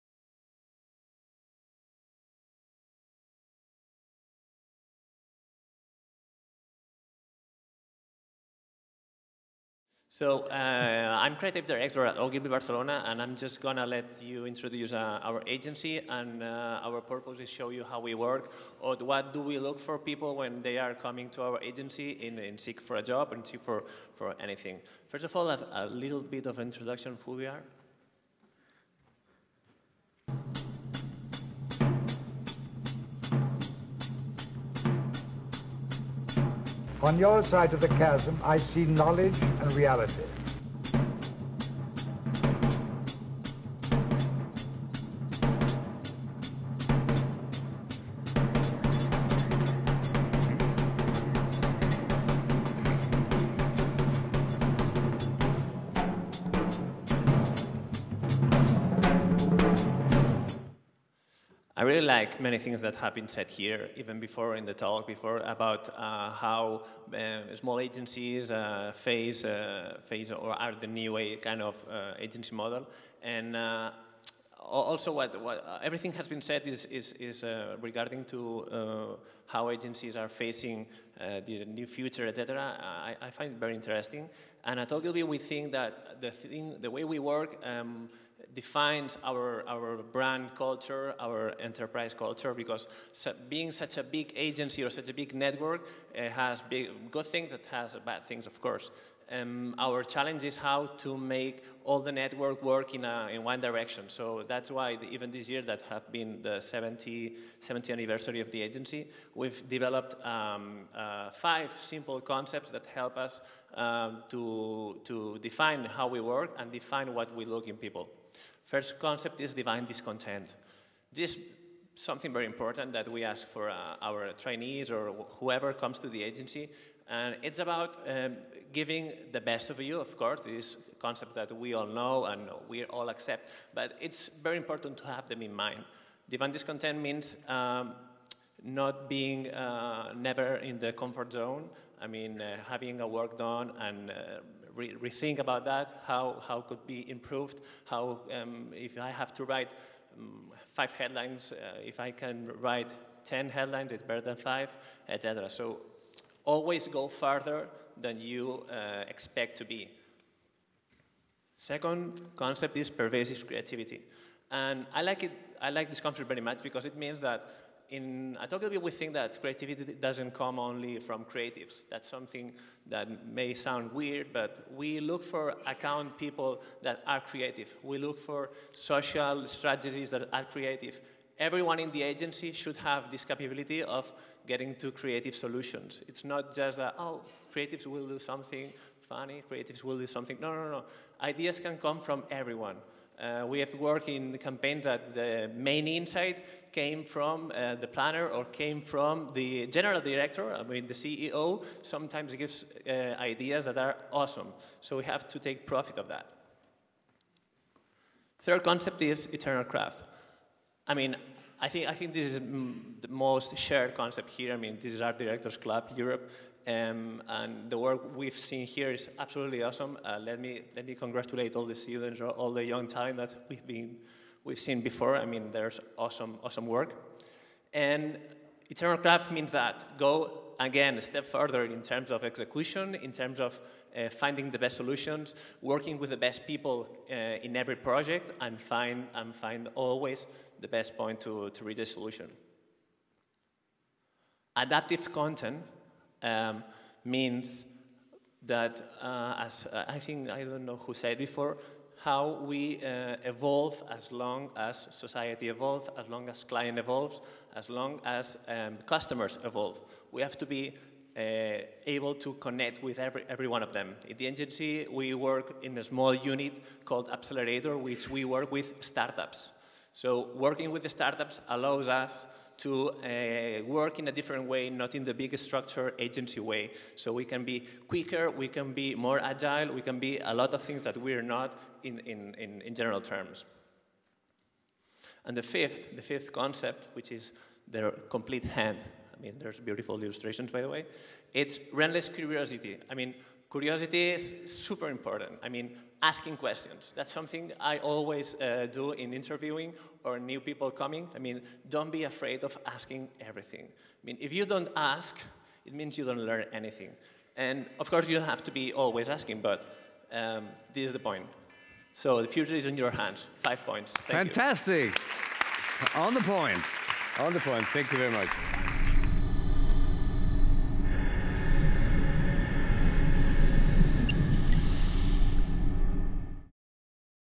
Jofre Banquells (Ogilvy Barcelona) - ADCE High Potentials 2018
As part of the ADCE European Creativity Festival 2018, the High Potentials session is an event where young European creative talents presented their best work to top agency creatives and HR managers, as well as the general public for a chance to find new career opportunities.

A challenging and inspiring session, High Potentials is designed to help creatives showcase their skills, make new acquaintances and ultimately, find irresistible jobs. Among the speakers are guests from the HR profession, who offer insight into the selection process, and a personal branding coach on hand to help individuals sell themselves better in a competitive industry.